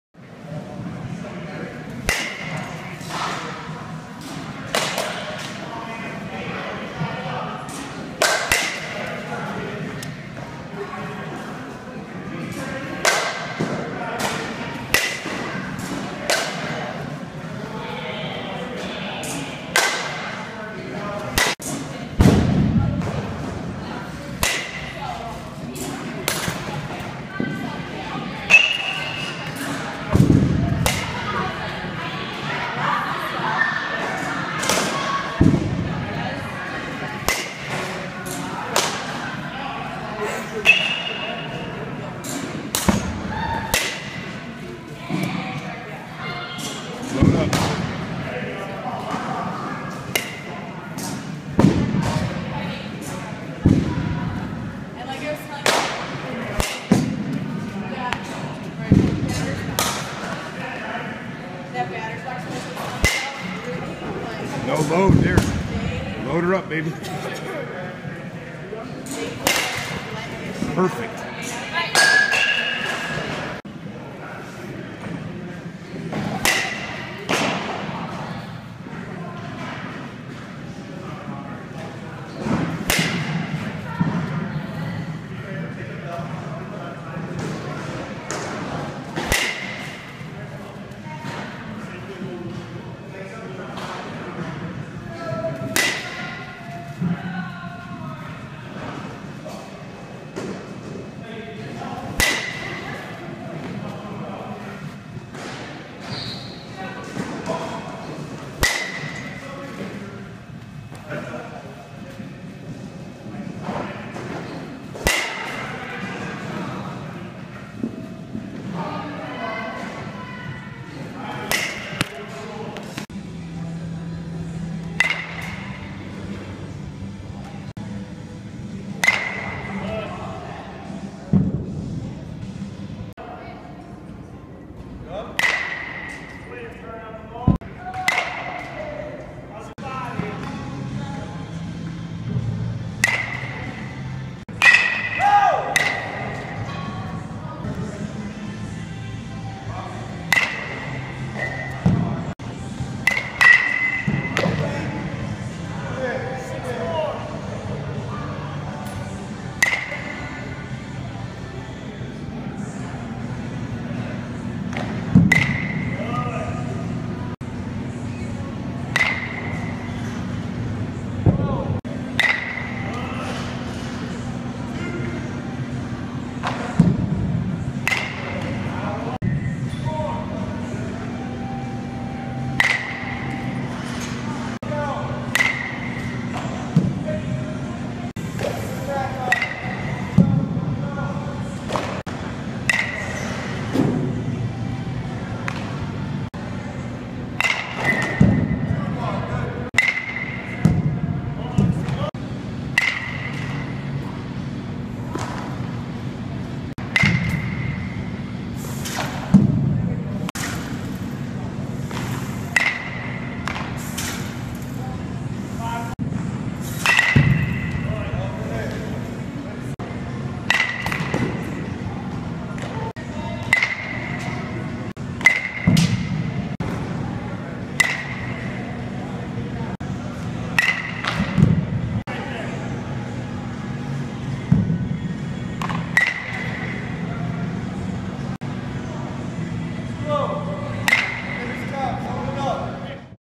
JTrain Training for 2015 Hitting
Jesse JTrain James training for the upcoming 2015 season - hitting